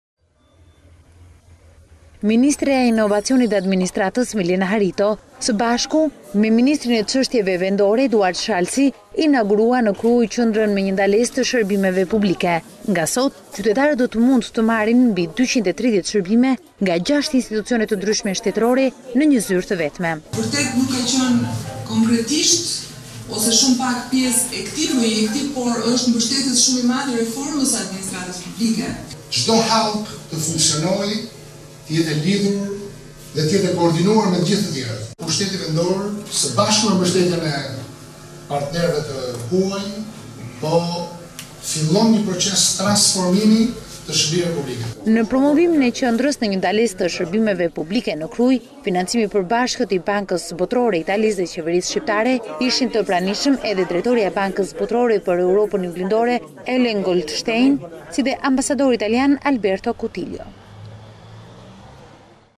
Ora News – Çelet qendra me një ndalesë, ofron 260 shërbime nga 6 institucione
Në Krujë është çelur qendra me një ndalesë, ku qytetarët do të mund të marrin rreth 230 shërbime nga 6 institucione shtetërore. Gjatë inaugurimit të qendrës, ministrja për Administratën, Harito dhe ministri për Pushtetin Vendor, Shalsi, theksuan se qëllimi i vetëm është përmirësimi i shërbimit ndaj qytetarëve.
Ministrja e Inovacionit dhe Administratës Milena Harito së bashku me ministrin e Pushtetit Vendor Eduart Shalsi, inauguruan në Krujë, qendrën me një ndalesë të Shërbimeve Publike. Nga sot qytetarët do të mund të marrin mbi 230 shërbime nga 6 institucione të ndryshme shtetërore, në një zyrë të vetme.
Harito: Për tregun ka qenë konkretisht ose shumë pak pjesë e këtij projekti por është mbështetës shumë i madh i reformës së administratës publike.
Shalsi: Cdo hallkë të funksionojë, të jetë e lidhur dhe të jetë e koordinuar me të gjithë të tjerët. Pushteti vendor, së bashku me mbështetjen nga partnerët e huaj po fillon një proces transformimi të shërbimeve publike.